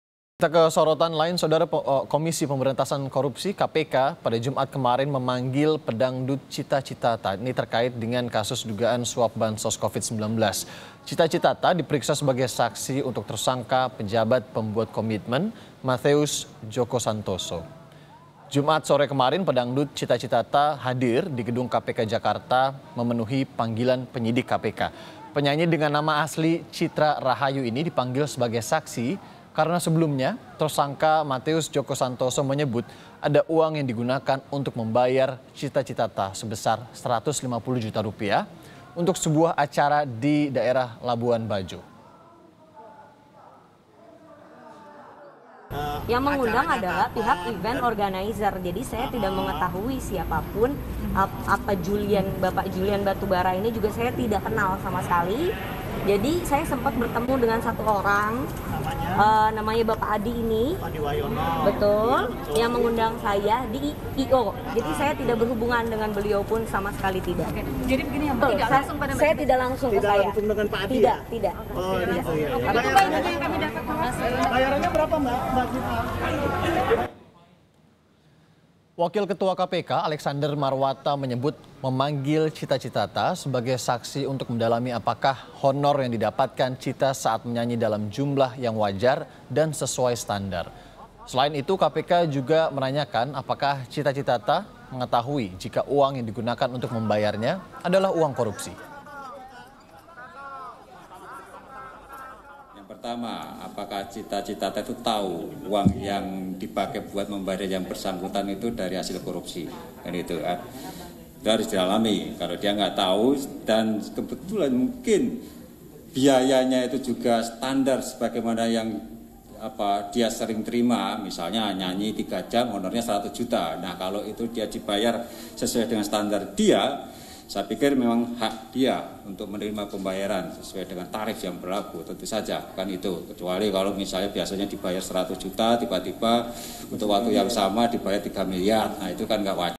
Kasus Korupsi Bansos, KPK Periksa Pedangdut Cita Citata
JAKARTA, KOMPAS.TV - Komisi Pemberantasan Korupsi (KPK), Jumat kemarin (26/03/2021) memanggil Penyanyi Dangdut Cita Citata terkait kasus dugaan suap bansos covid-19.

Cita Citata diperiksa sebagai saksi untuk tersangka pejabat pembuat komitmen Matheus Joko Santoso.

Jumat sore, Penyanyi Dangdut Cita Citata hadir di Gedung KPK Jakarta memenuhi panggilan penyidik.

Penyanyi dengan nama asli Citra Rahayu ini dipanggil sebagai saksi, karena sebelumnya tersangka Matheus Joko Santoso menyebut ada uang yang digunakan untuk membayar Cita Citata sebesar Rp 150 juta untuk sebuah acara di Labuan Bajo.

Wakil Ketua KPK, Alexander Marwata menyebut memanggil Cita Citata sebagai saksi untuk mendalami apakah honor yang didapatkan cita saat menyanyi dalam jumlah yang wajar dan sesuai standar.

Selain itu, KPK juga menanyakan apakah Cita Citata mengetahui jika uang yang digunakan untuk membayarnya adalah uang korupsi.

"Yang mengundang adalah pihak event organizer, jadi saya tidak mengetahui siapapun dan saya tidak kenal Bapak Juliari Batubara sama sekali." ujar Penyanyi Dangdut Cita Citata.